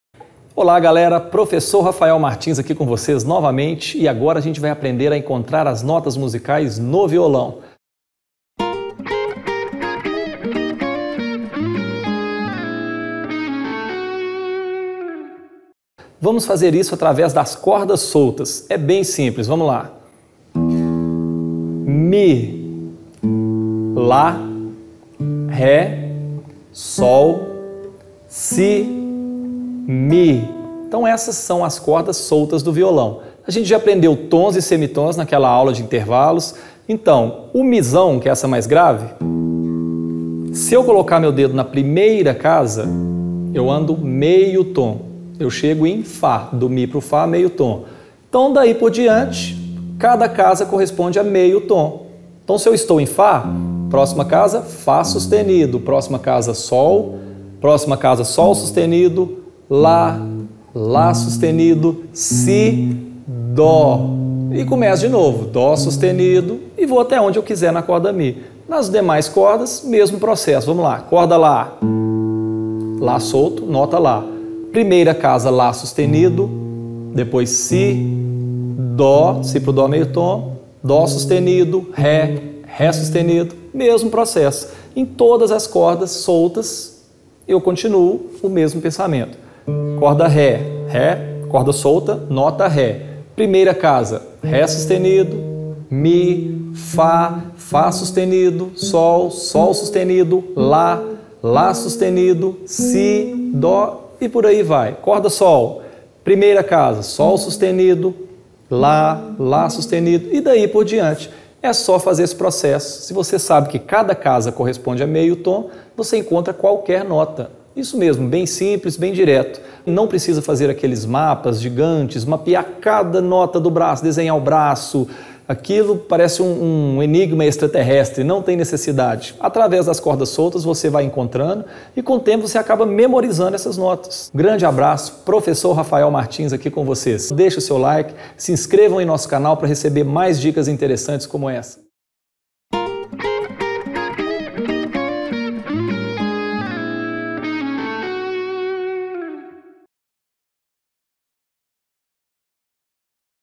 Notas no violão
Nesse vídeo, ensino como encontrar qualquer nota no braço do violão com um cálculo simples. Usando uma estrutura que substitui mapas mirabolantes